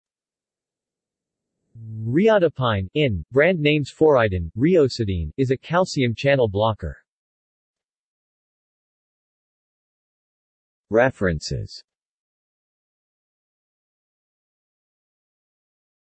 Riodipine | Wikipedia audio article
This is an audio version of the Wikipedia Article:
Riodipine

SUMMARY
Riodipine (INN) (brand names Foridon, Ryosidine) is a calcium channel blocker.